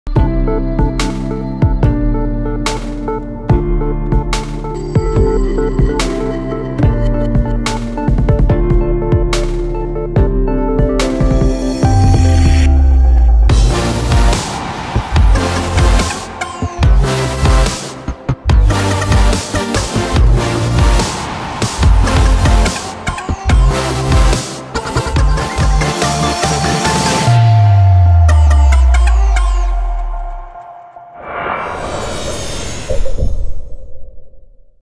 OLIGHT FR i1R2 - Lampe Porte-clé
OLIGHT i1R2 EOS - LAMPE PORTE-CLÉ RECHARGEABLE AU DESIGN COMPACT

La lampe de poche i1R 2 EOS est une version améliorée de la i1R EOS. Elle utilise comme nouvelle source de lumière une LED CSP (Chip Scale Packaging) qui peut briller par 5 côtés, la luminosité de sortie atteignant ainsi 150 lumens maximum.